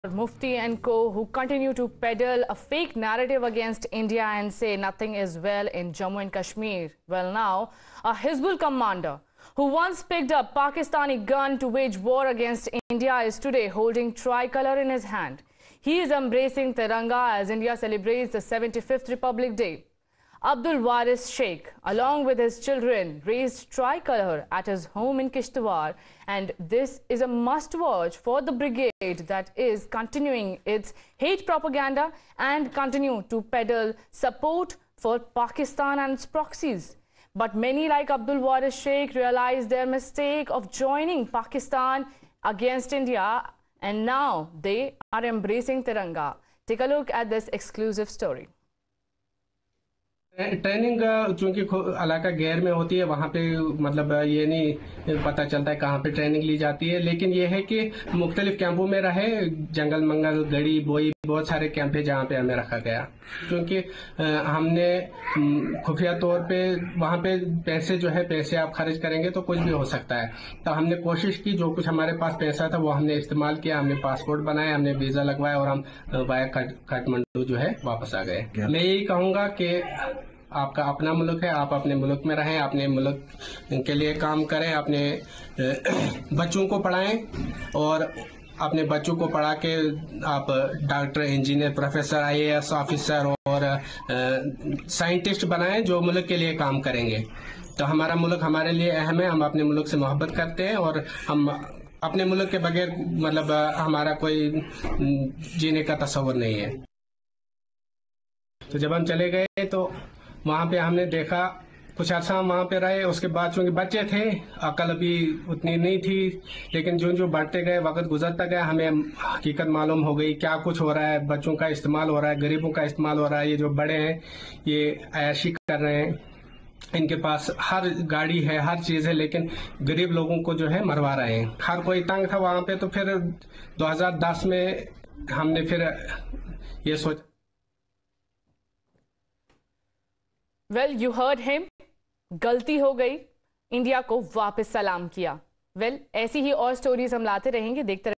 75th Republic Day News |'I Had Made Mistake...', Says Hizbul Commander After He Embrases Indian Flag
As per the latest news on Times Now, Hizbul Commander who once picked up the Pakistani flag to wage war against India is today holding tricolour in his hand. He is embracing the flag as India celebrates its 75th Republic Day. Abdul Waris Sheikh along with his children raised tricolor at his home in Kishtwar.